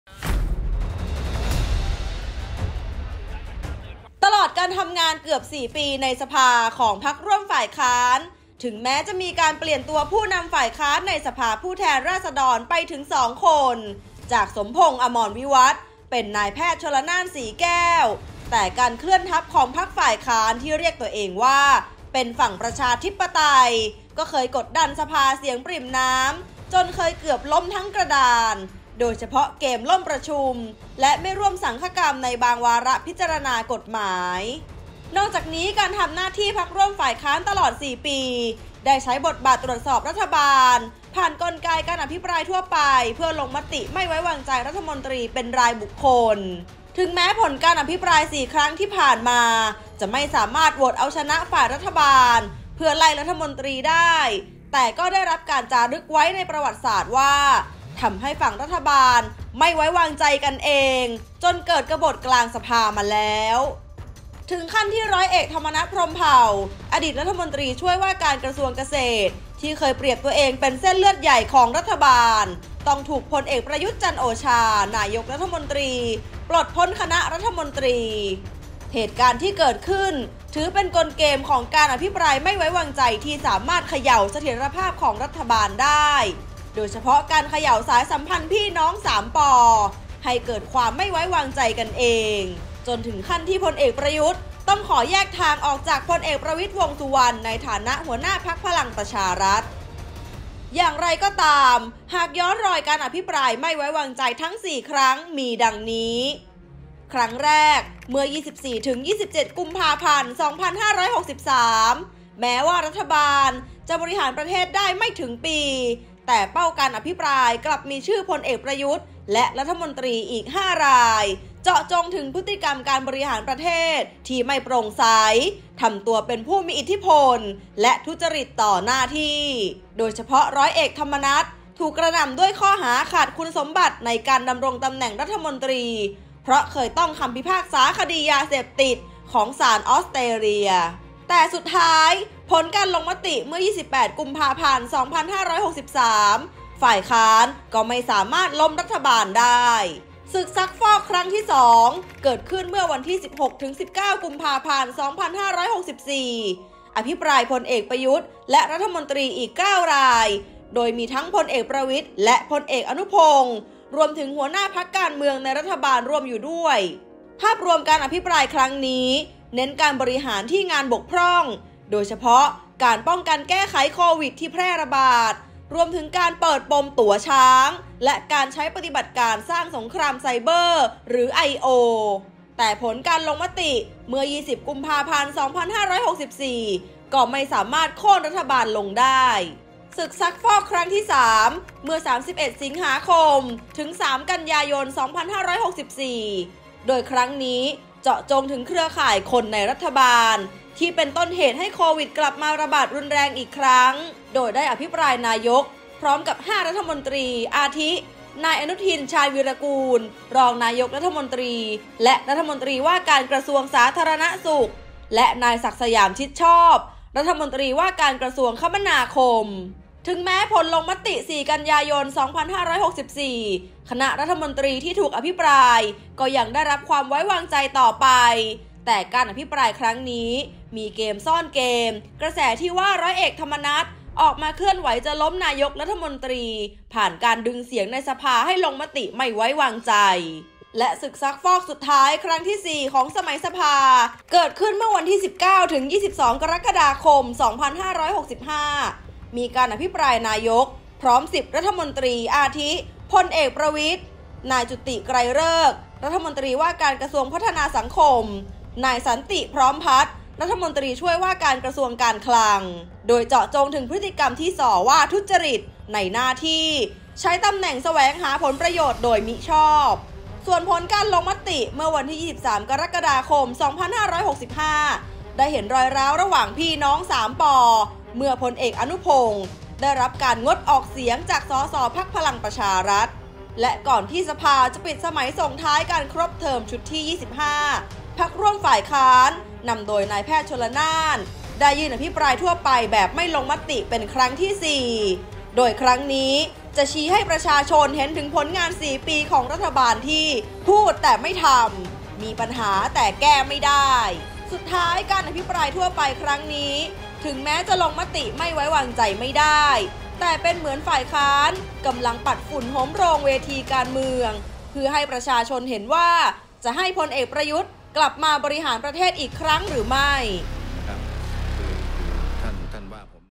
ตอกลิ่ม 3 ป. ก่อกบฎรัฐบาล !!
ผลการอภิปราย 4 ครั้งได้รับการจารึกไว้ว่า ทำให้ “ฝั่งรัฐบาล​" ไม่ไว้วางใจกันเอง จนเกิดกบฎกลางสภาฯ !!!